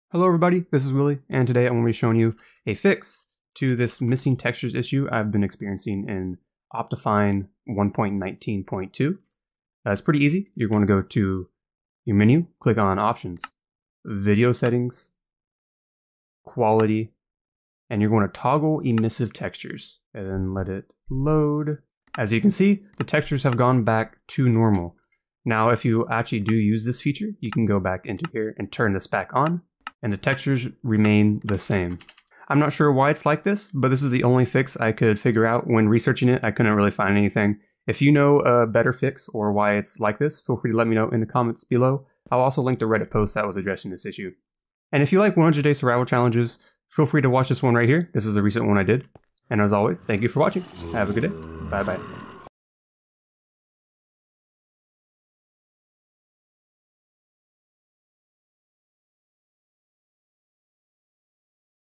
How To Fix Missing Textures In Optifine 1.19.2+ [Blocks Are Purple?]
Today I show a quick fix for missing textures in optifine 1.19.2.